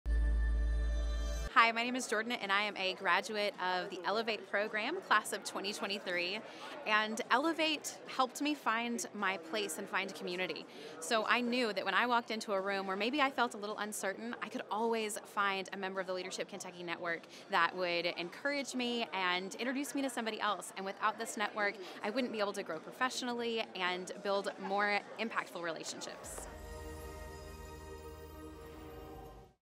Jordyne Carmack, ELEVATE Class of 2023
Leadership Kentucky Founders, Board Members, and Alumni shared the impact of Leadership Kentucky at the 40th Anniversary Alumni Luncheon. Brief videos showcase their experiences and will be featured throughout this year in celebration of the 40th Anniversary. This video in the series features Jordyne Carmack, ELEVATE Class of 2023.